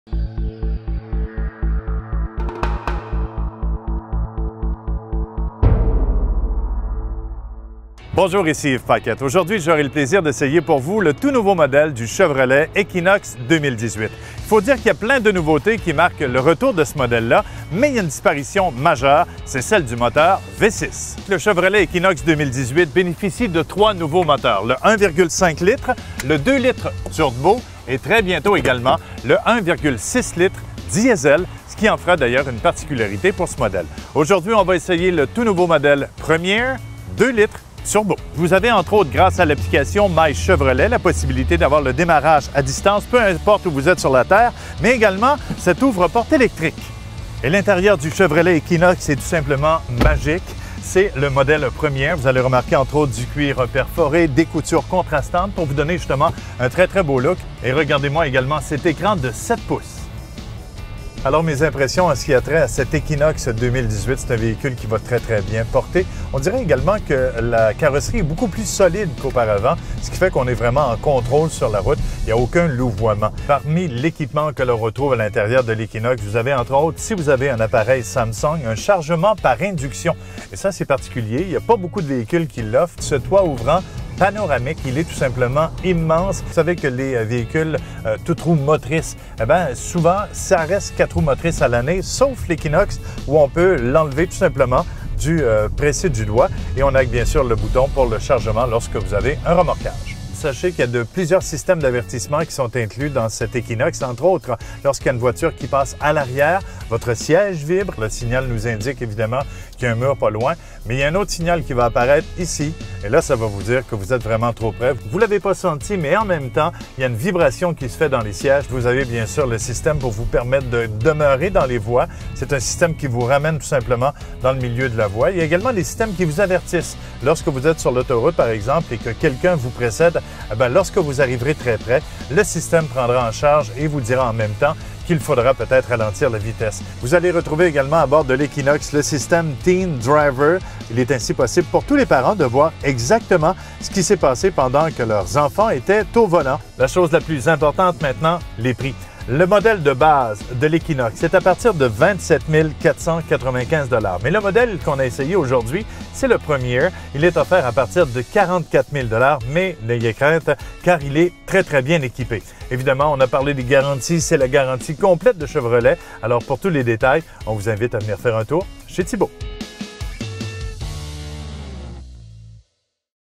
Essai routier du Chevrolet Equinox 2018 - EstriePlus.com
Notre chroniqueur automobile Yves Paquette essaie pour vous le tout nouveau modèle du Chevrolet Equinox 2018. Il vous explique aussi les particularités et les forces de ce véhicule